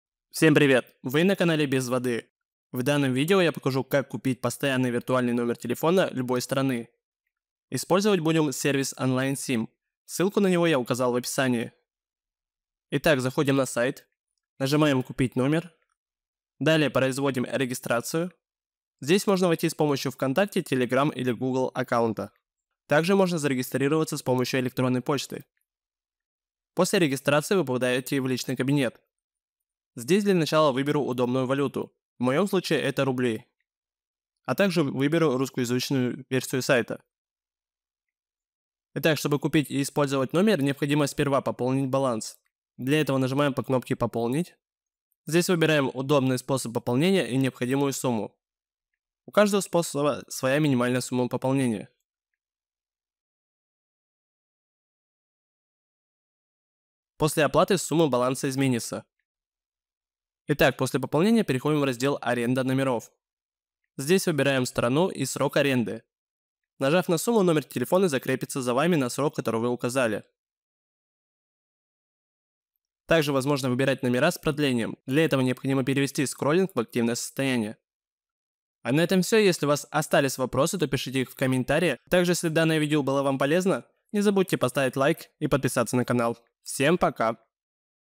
Как купить Постоянный Виртуальный Номер
Всем привет друзья! В данном видео ВЫ узнаете Где купить Постоянный Виртуальный Номер